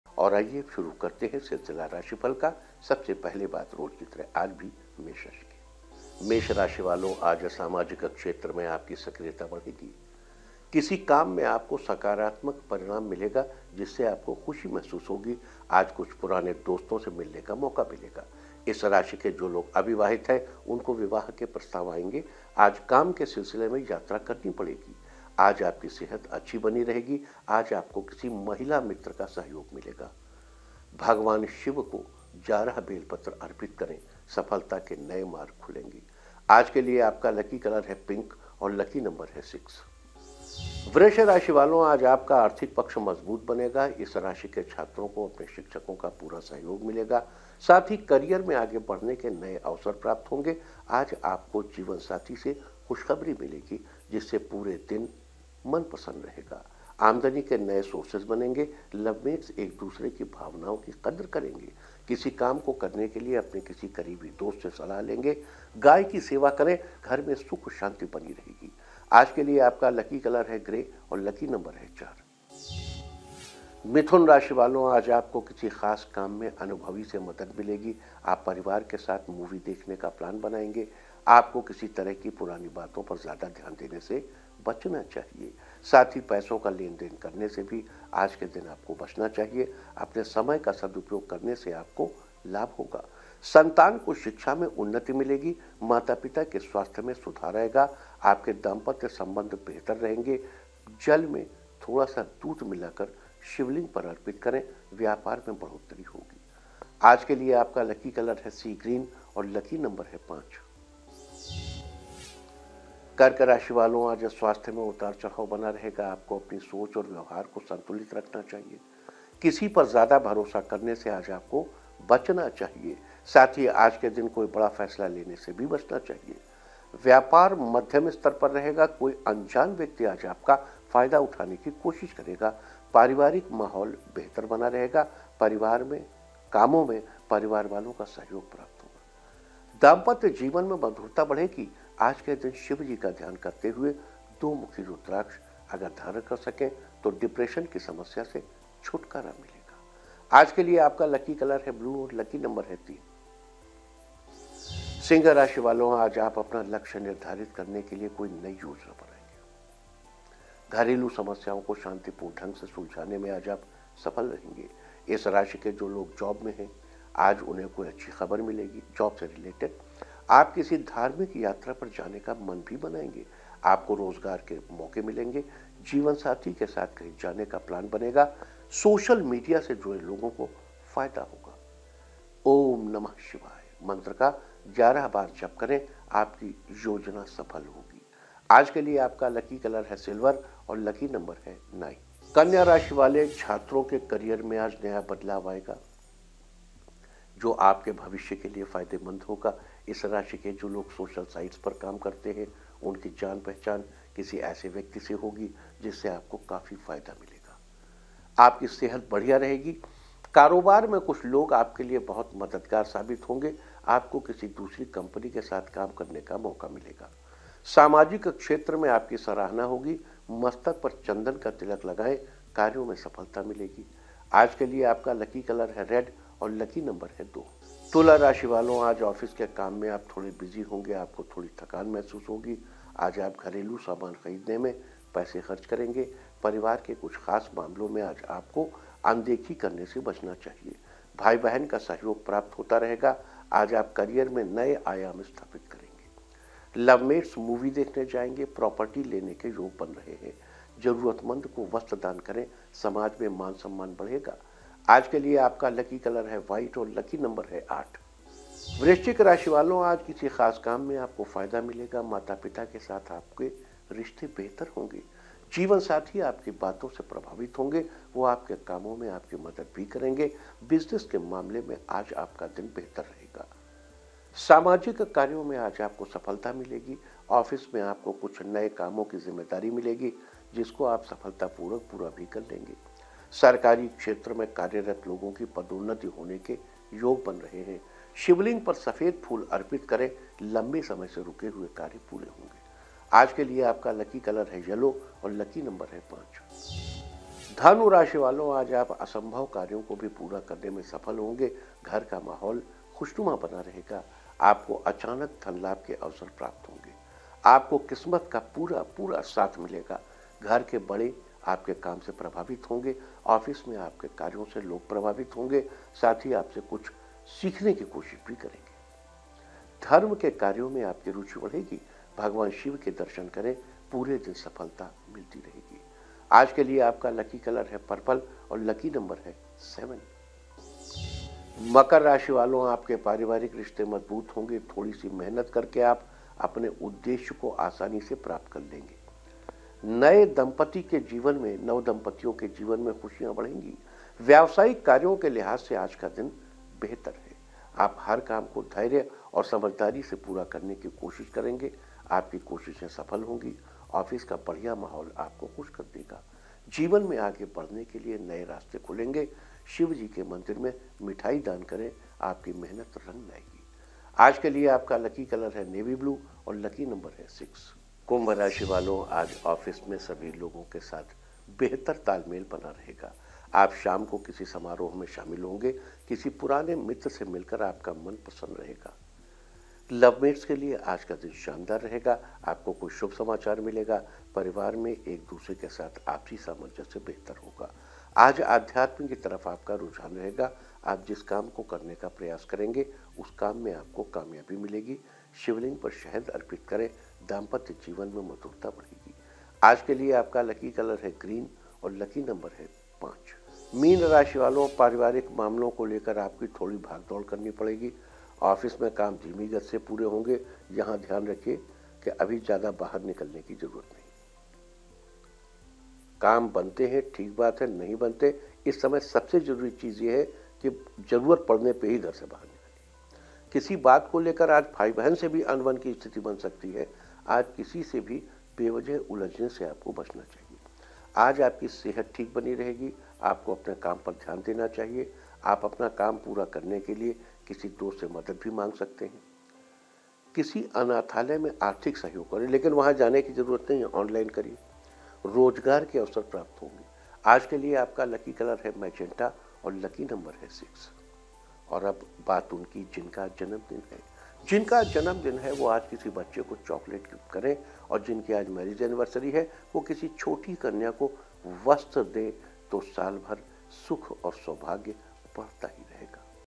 राशिफल: वृष राशि वालों का आज आपका आर्थिक पक्ष बनेगा मजबूत
वृष राशि - वालों आज आपका आर्थिक पक्ष मजबूत बनेगा |इस राशि के छात्रों को अपने शिक्षकों का पूरा-पूरा सहयोग मिलेगा | साथ ही करियर में आगे बढ़ने के नए अवसर प्राप्त होंगे | आज आपको जीवनसाथी से खुशखबरीमिलेगी, जिससे पुरे दिन मन प्रसन्न रहेगा | आय के नये स्त्रोत बनेगे